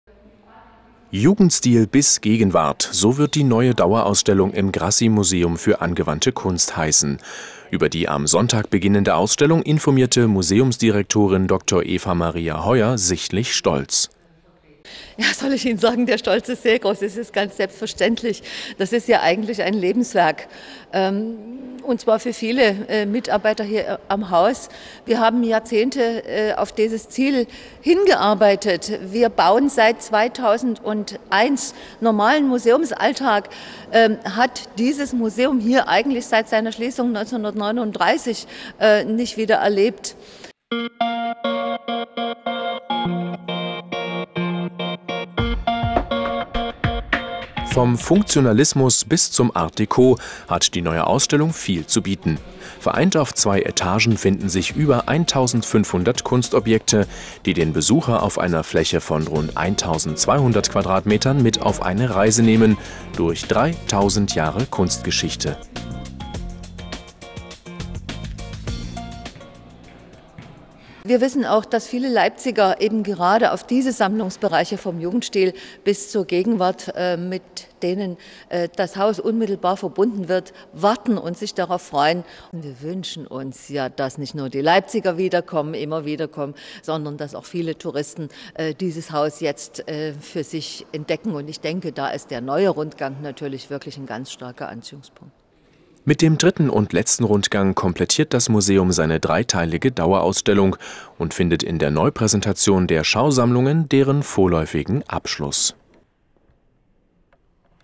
»Jugendstil bis Gegenwart«
neue Dauerausstellung im GRASSI Museum

»Jugendstil bis Gegenwart« - so wird die neue Dauerausstellung im GRASSI Museum für Angewandte Kunst heißen. Über die am Sonntag beginnende Ausstellung informierte Museumsdirektorin Dr. Eva Maria Hoyer sichtlich stolz.

»Der Stolz ist sehr groß, das ist ganz selbstverständlich. Für viele Mitarbeiter hier am Haus ist das eigentlich ein Lebenswerk. Wir haben Jahrzehnte lang auf dieses Ziel hingearbeitet. Wir bauen nun schon seit 2001. Einen normalen Museumsalltag hat dieses Museum seit seiner Schließung 1993, eigentlich nie wieder erlebt«, berichtete die stolze Museumsdirektorin des GRASSI Museums für Angewandte Kunst, Dr. Eva Maria Hoyer

Vom »Funktionalismus« bis zum »Art déco« hat die neue Ausstellung viel zu bieten. Vereint auf zwei Etagen finden sich über 1.500 Kunstobjekte, die den Besucher auf einer Fläche von rund 1.200 m² auf eine Zeitreise mitnehmen - durch 3.000 Jahre Kunstgeschichte entführen.

»Wir wissen das besonders viele Leipziger auf das Thema Jugendstil, was auch unmittelbar mit dem Haus verbunden wird, warten und sich darauf freuen. Wir würden uns wünschen das nicht nur die Leipziger immer wieder kommen, sondern gerade auch Touristen das Haus für sich entdecken. Ich denke da ist der neue Rundgang natürlich ein ganz starker Anziehungspunkt«, ergänzte Hoyer

Mit dem dritten und letzten Rundgang komplettiert das Museum seine dreiteilige Dauerausstellung und findet mit der Neupräsentation der Schausammlungen vorläufig ihren Abschluss.

(Vielen Dank für den freundlichen Hinweis auf den Schnittfehler im O-Ton! Wir haben den Fehler gleich korrigiert.)